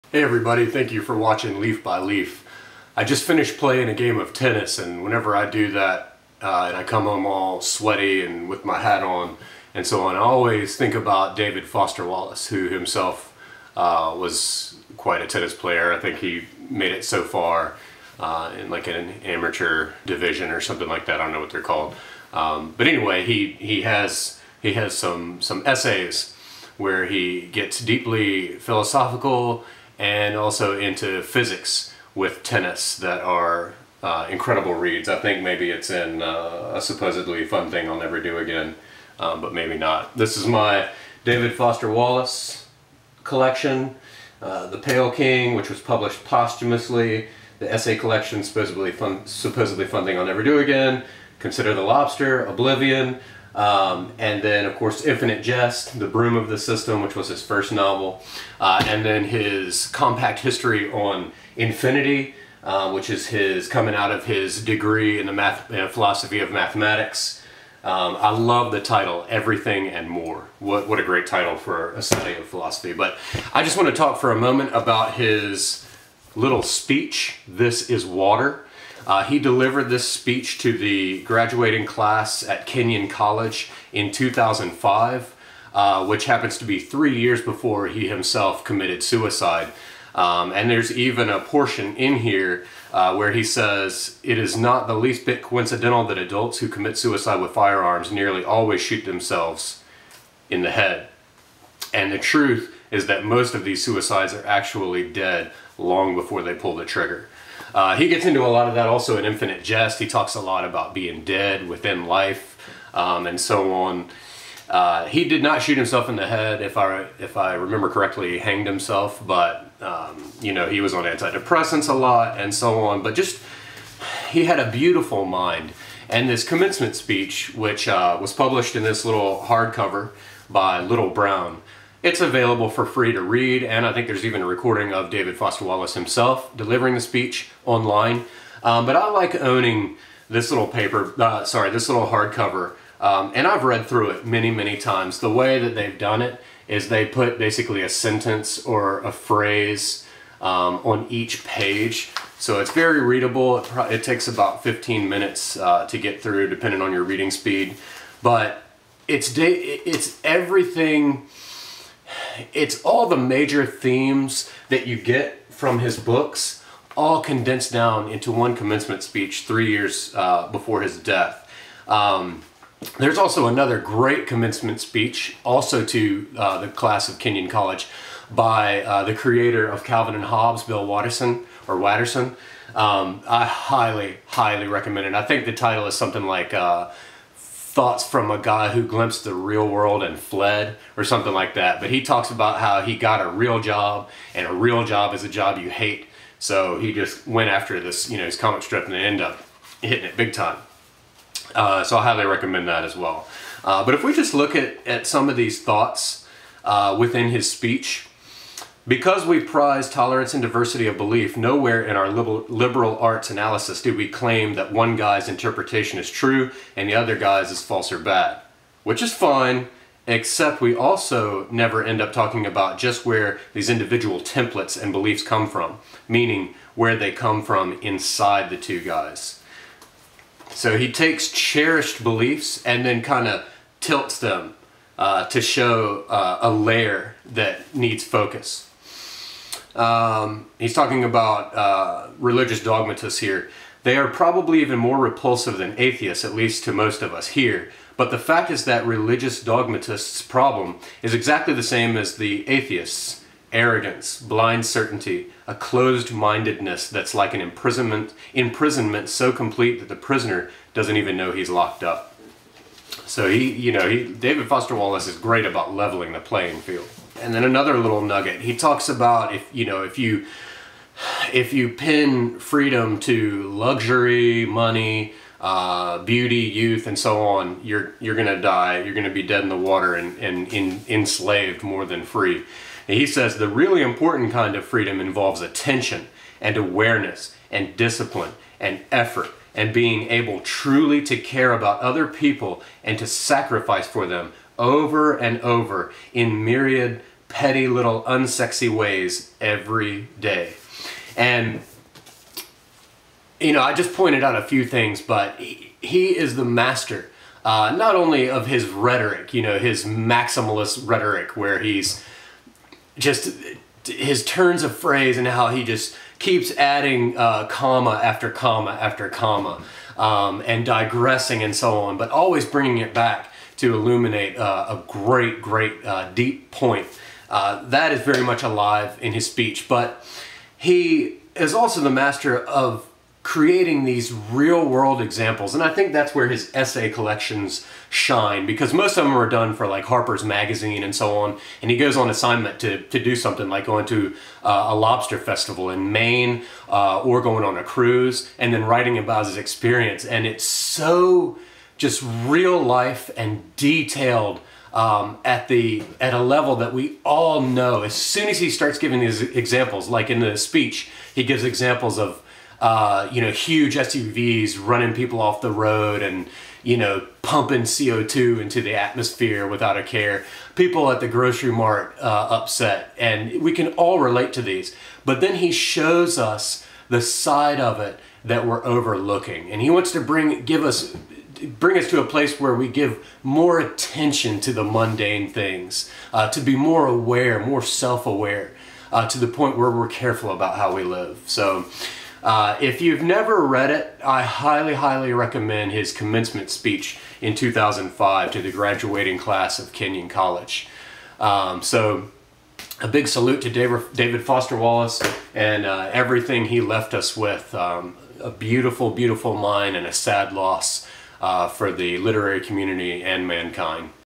THIS IS WATER by David Foster Wallace
After a vigorous tennis match this morning, I had David Foster Wallace on my mind. Here are some thoughts on his 2005 commencement speech at Kenyon College.